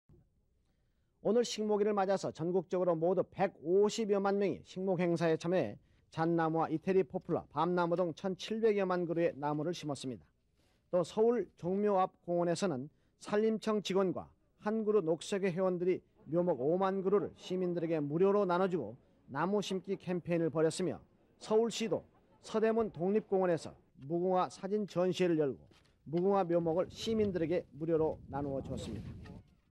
[신긔방긔~] 식목일 맞아 150만명 식목행사 참가!!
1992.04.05 식목일 맞아 150만명 식목행사 참가

▶대한민국 역대급 캐씁쓸한 사건들 ‘한숨밖에안나와 NEWS‘
▶대한민국 역대급 훈훈한 사건들 ‘감동눈물쓰나미 NEWS'